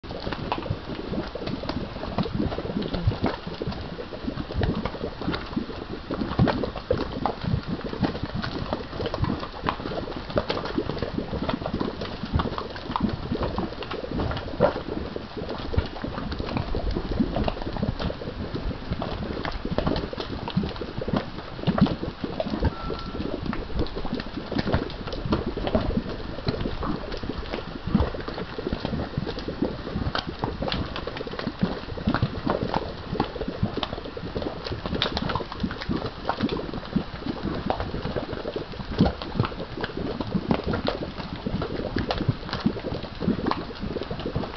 More Boiling Mud
Another view of the boiling mud in Rincon de la Vieja National Park in northern Costa Rica